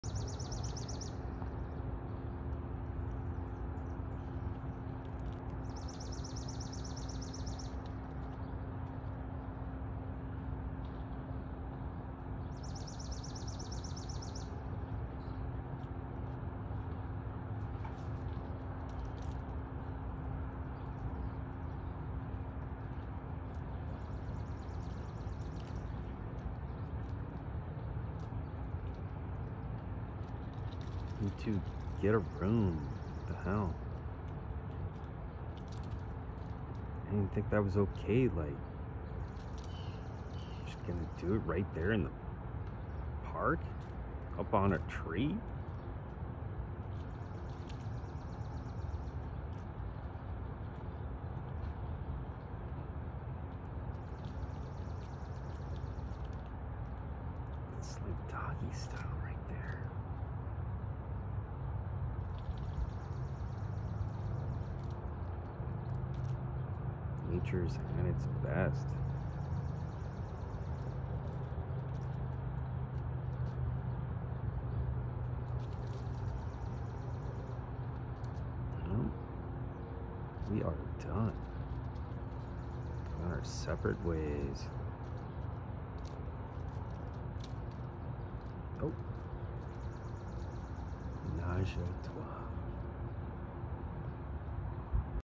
Squirrel Sex
I came across some squirrel going at it in the park!